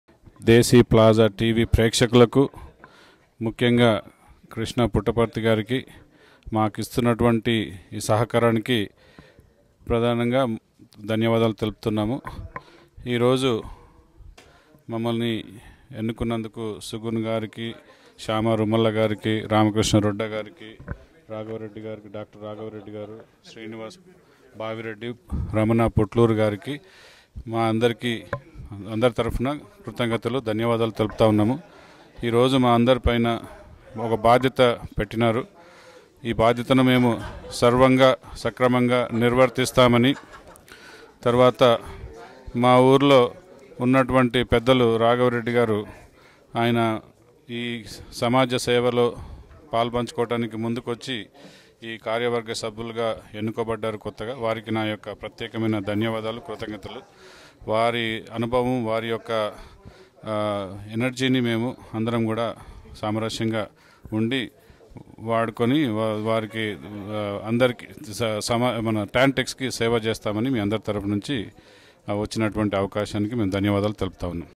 Guram Srinivas Reddy Speaking at TANTEX BOT Election meeting
TANTEX 2016 EC elections Meet held on January 2nd 2015 at hilltop indian Cuisine.

Please Do Visit our Websites

Download our Desiplaza Tv apps from

Download our MastiTime Radio apps from